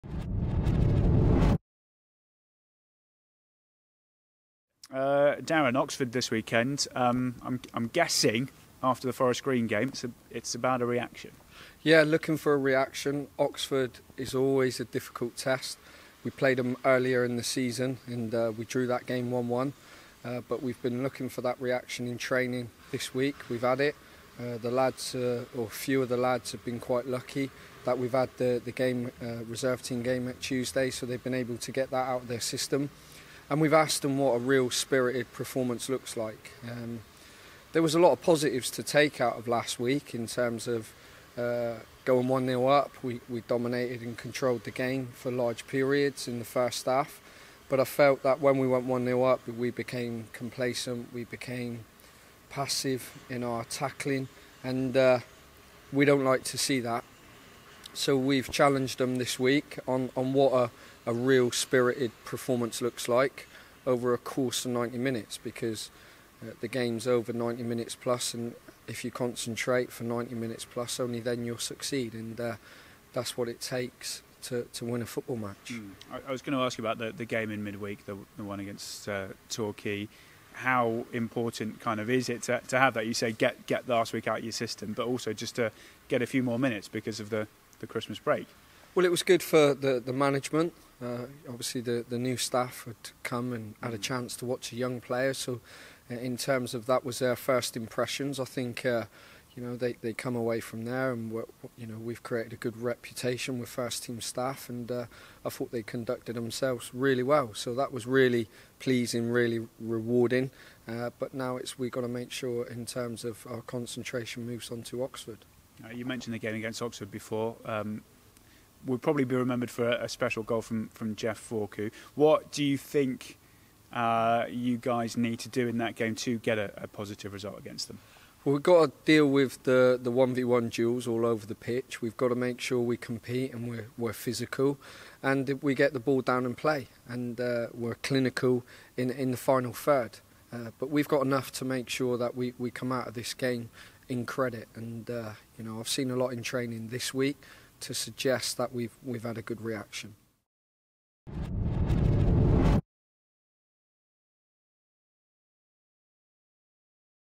Interview | Darren Way Previews Oxford United
Darren Way previews this weekend's EFL Youth Alliance clash against Oxford United.